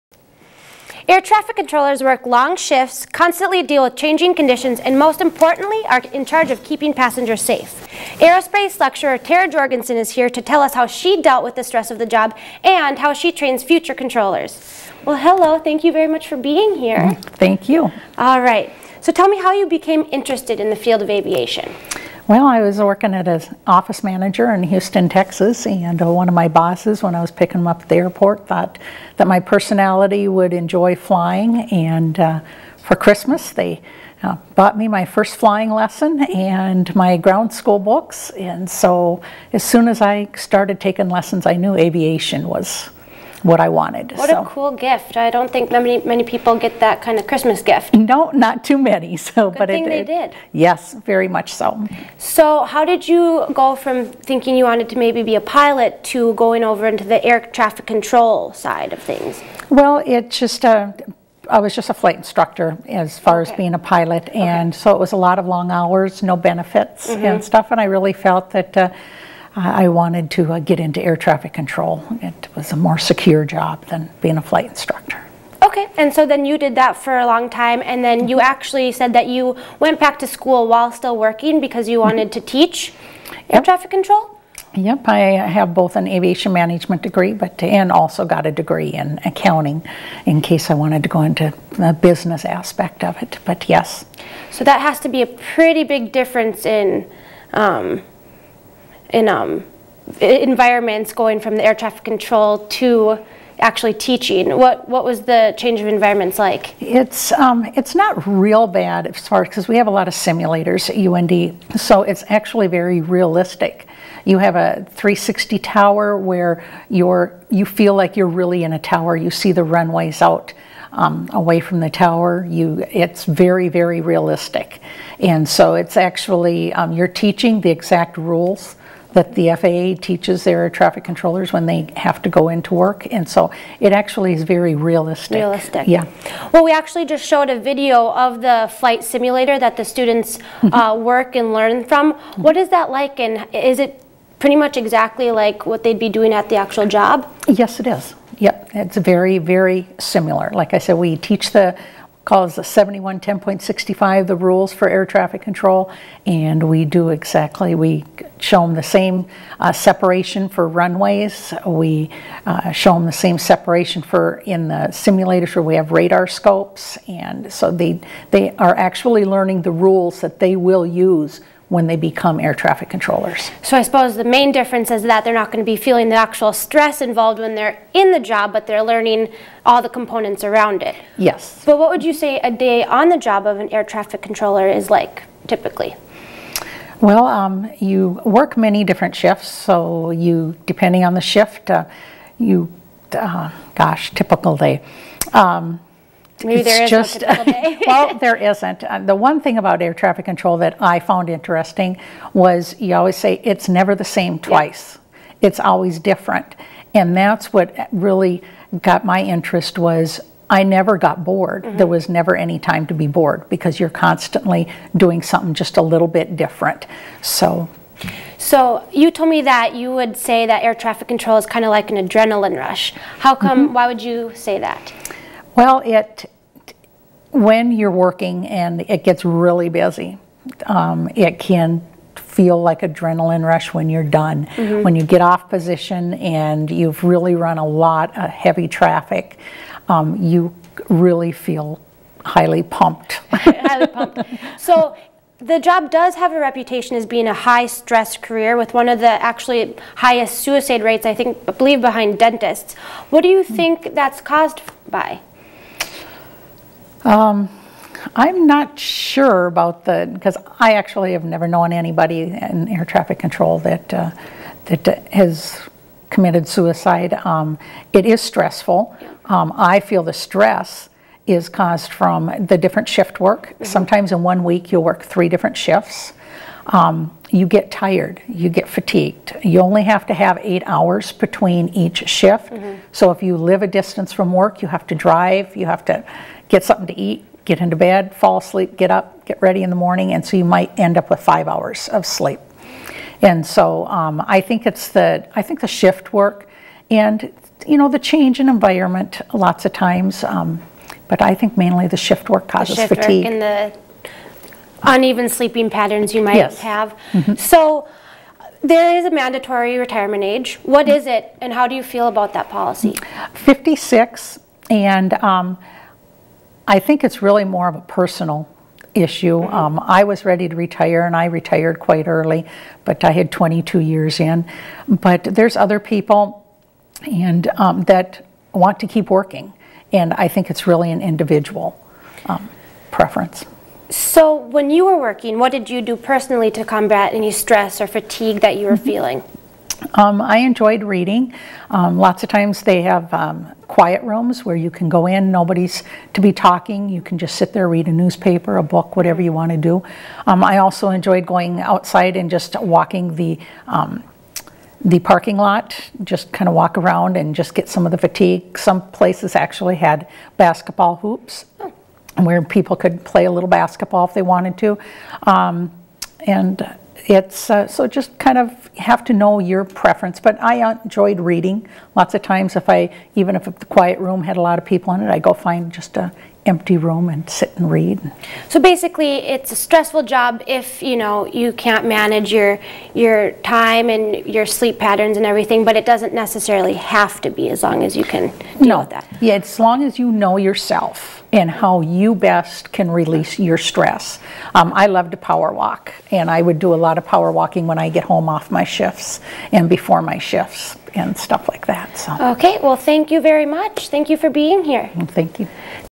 Terra Jorgenson | Air Traffic Control Professor
Terra Jorgenson is a retired pilot and air traffic controller.  She now teaches at the University of North Dakota.  She tells us about the career of air traffic control and how its stressful nature can be dealt with in a variety of ways.